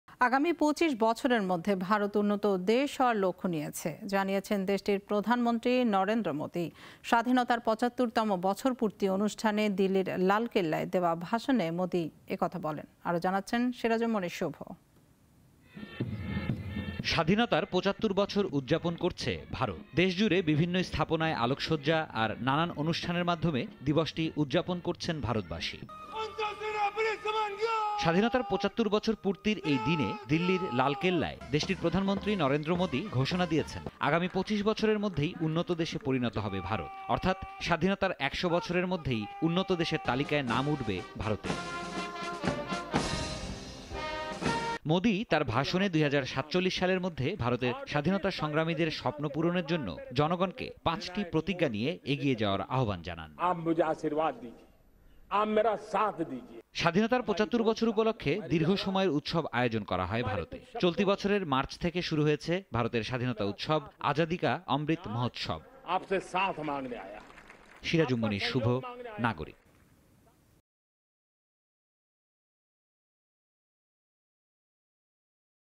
২৫ বছরে উন্নত দেশ হবে ভারত, স্বাধীনতা দিবসে মোদি | India | Modi | Nagorik News
২৫ বছরে উন্নত দেশ হবে ভারত, স্বাধীনতা দিবসে মোদি...